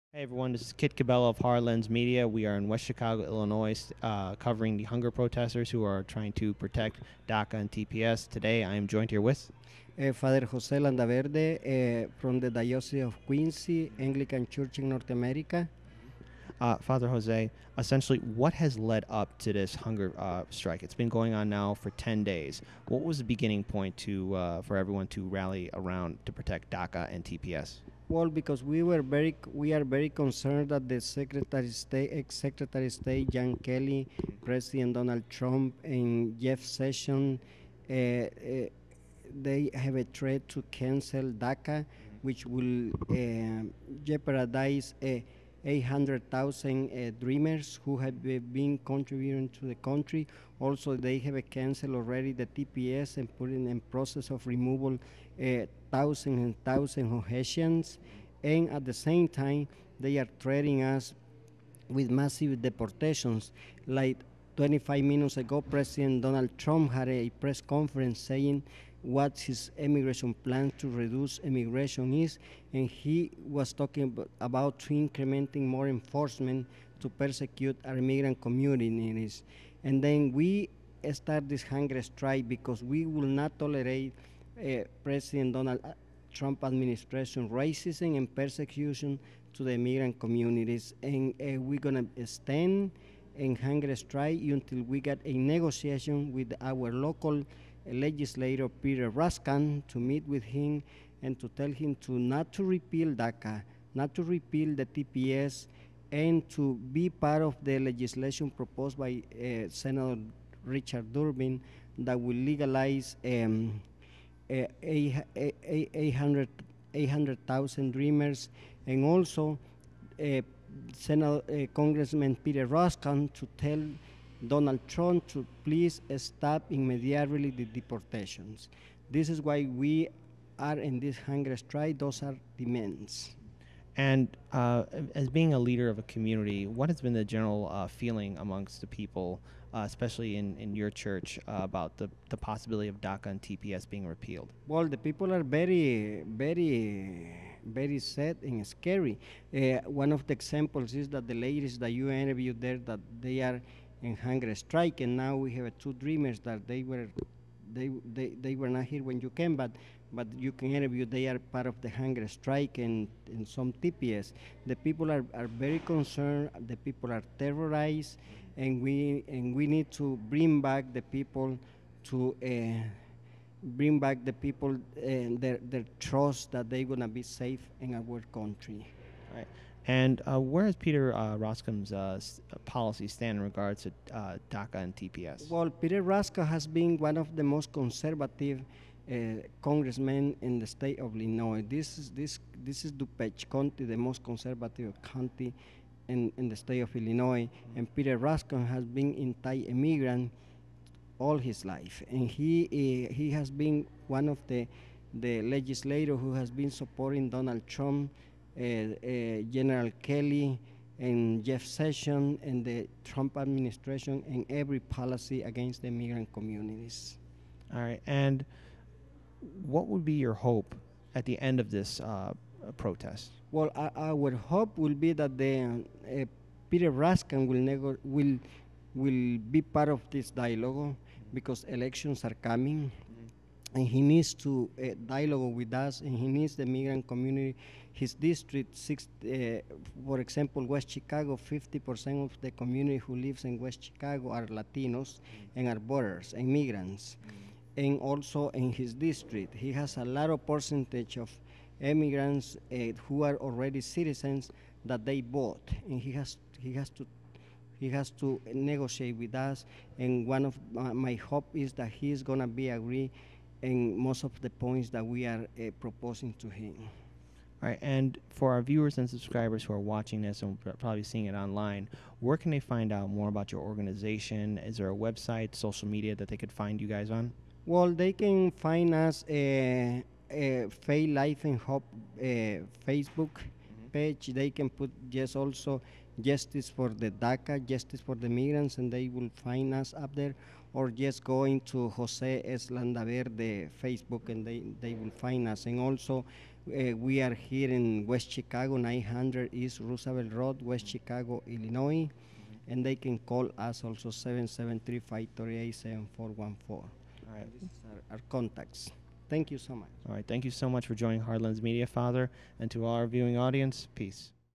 On tenth day of the hunger strike, Father José Landaverde speaks with HLM
On tenth day of the hunger strike, Father José Landaverde speaks with Hard Lens Media about the resistance to the repeal of DACA, TPS, and Peter Roskam.

8-2-2017
for more content